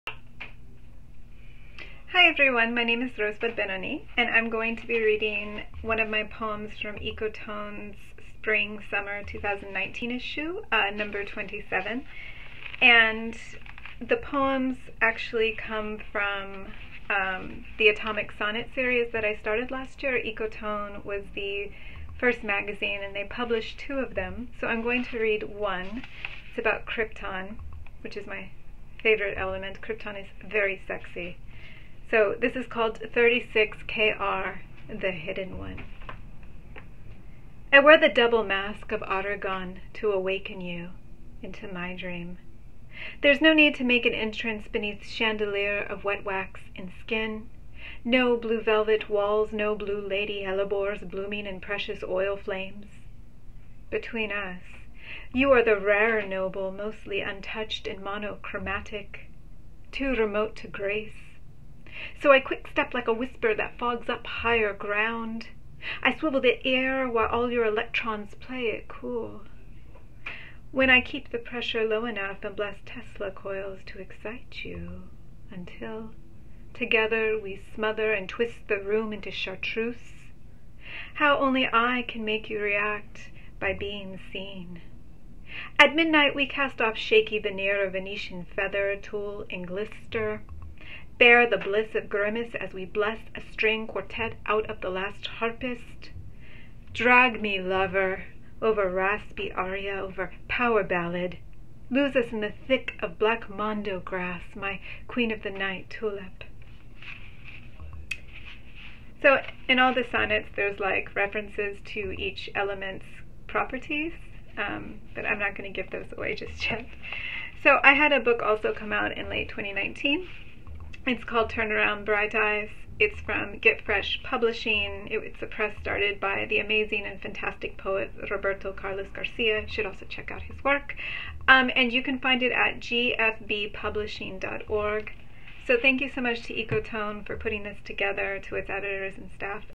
Read with Ecotone: Rosebud Ben-Oni
A number of Ecotone contributors, like many writers, have had book launches interrupted by the 2020 coronavirus pandemic. We want to celebrate these writers and all the work they’ve put into their books. We’ve asked them to share a video of themselves reading their work from Ecotone, and to tell us a little about their books as well.

Rosebud Ben-Oni published the first two poems in her Atomic Sonnets series in Issue 27 Her book, turn around, Brxght Xyxs, is available from Get Fresh Books,

Rosebud Ben-Oni is the winner of the 2019 Alice James Award for If This Is the Age We End Discovery, forthcoming in 2021; her second collection, turn around, BRXGHT XYXS, was selected as Agape Editions’ Editor’s Choice, and was first published in 2019. Her poem “Poet Wrestling with Angels in the Dark” was commissioned by the National September 11 Memorial and Museum in New York. She writes for the Kenyon Review blog.